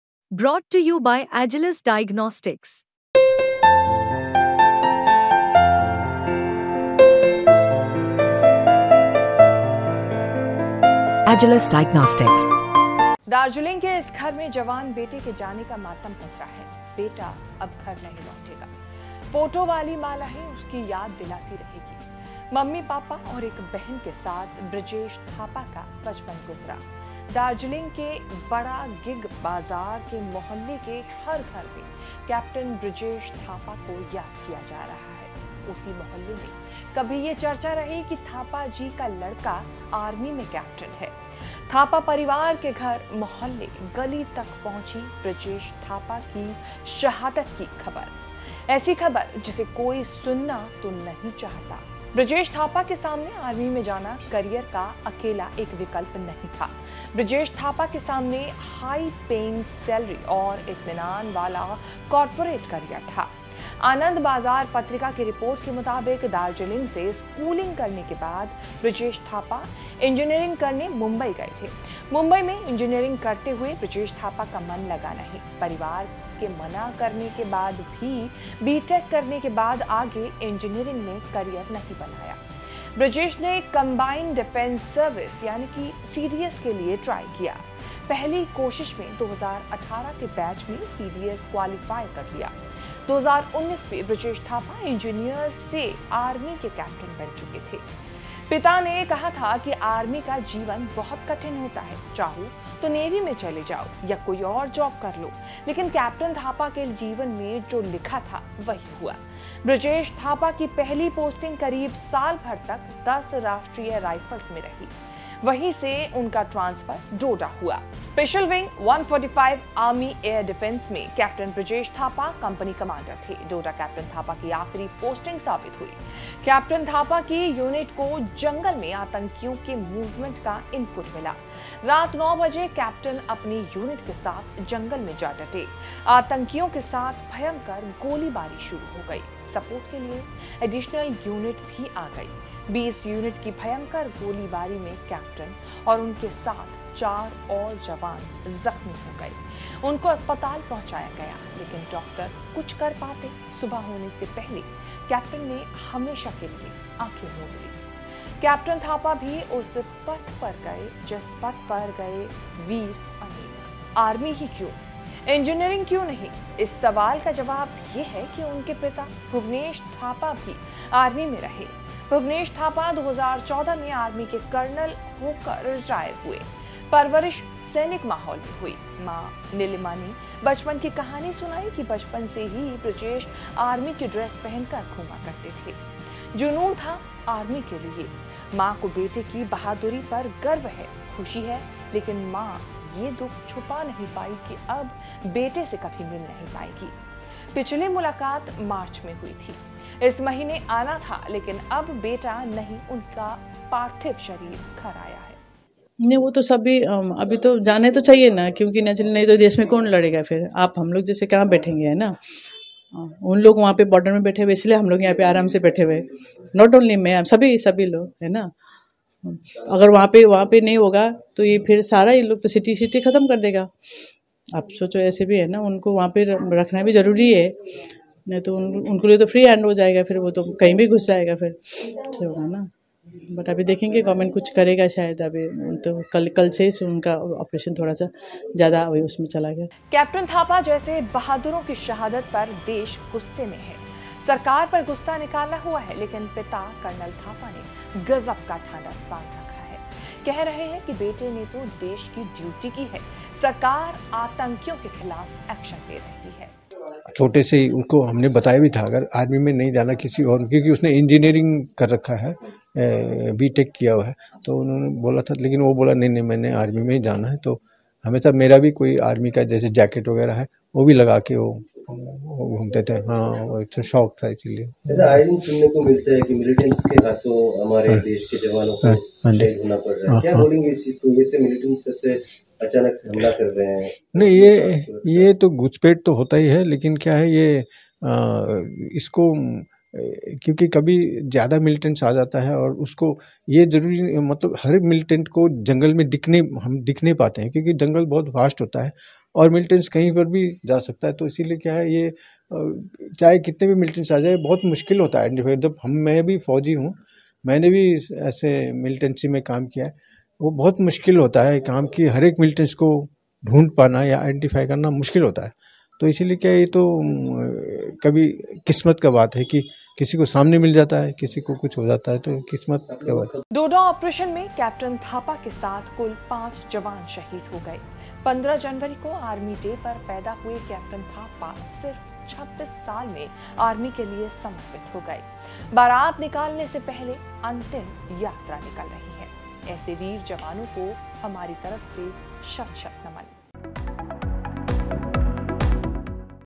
Capt. Brijesh Thapa की वो कहानी जिसने सुना रह गया दंग! बिलखती मां ने बताया सब कुछ...
दार्जिलिंग के इस घर में जवान बेटे के जाने का मातम पसरा है. बेटा अब घर नहीं लौटेगा. फोटो वाली माला ही उसकी याद दिलाती रहेगी. मम्मी-पापा और एक बहन के साथ बृजेश थापा का बचपन गुजरा. दार्जिलिंग के बड़ा गिंग बाजार के मुहल्ले के हर घर में कैप्टन बृजेश थापा को याद किया जा रहा है उसी मुहल्ले में कभी ये चर्चा रही कि थापाजी का लड़का आर्मी में कैप्टन है. थापा परिवार के घर, मोहल्ले, गली तक पहुंची ब्रजेश थापा की शहादत की खबर. ऐसी खबर जिसे कोई सुनना तो नहीं चाहता. बृजेश थापा के सामने आर्मी में जाना करियर का अकेला एक विकल्प नहीं था. बृजेश थापा के सामने हाई पेइंग सैलरी और इत्मीनान वाला कॉरपोरेट करियर था. लेकिन उन्होंने देश भक्ती को चुना और फिर जो हुआ वो सबकी आंखें नम कर गया...